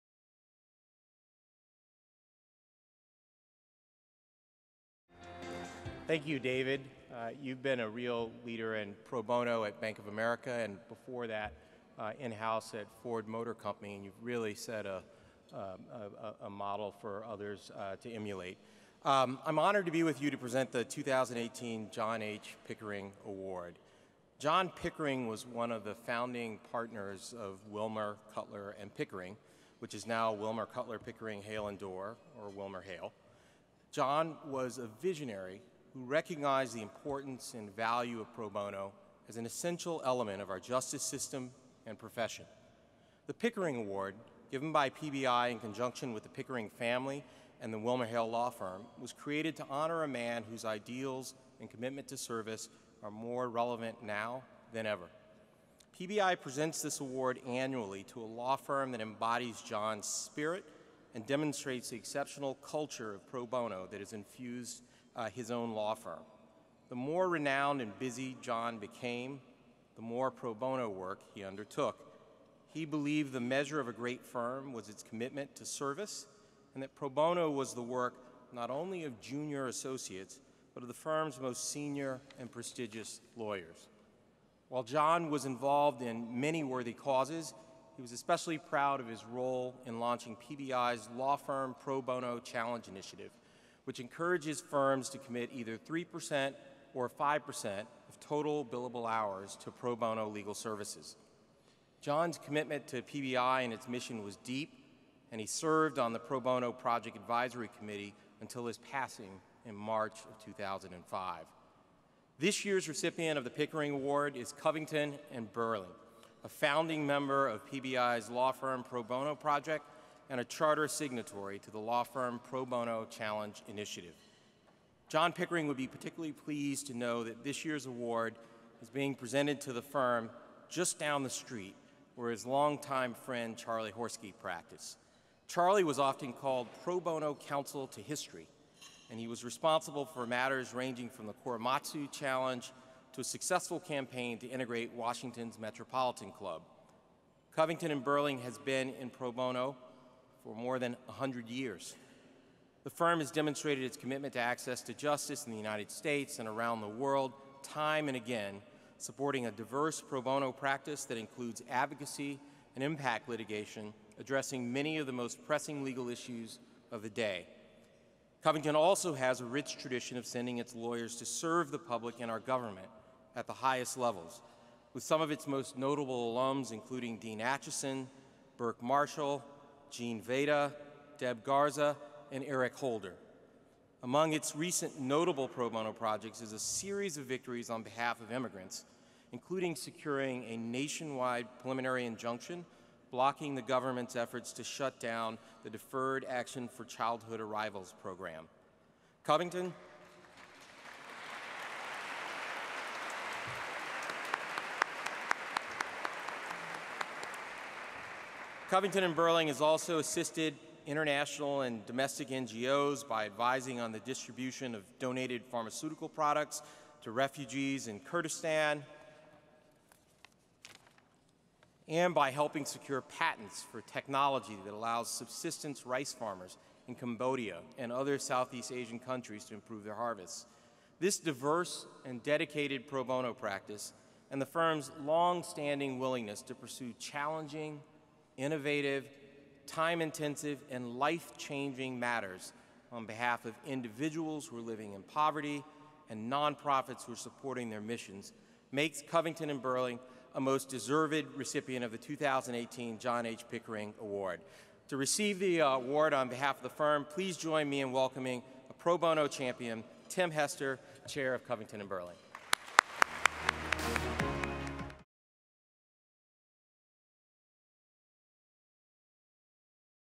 PBI 2018 Annual Dinner [04/13] - Reginald Brown, WilmerHale
Pro Bono Institute (PBI) hosted its 2018 Annual Dinner, themed “Transforming Pro Bono — Together,” on October 4 at Gotham Hall in New York City.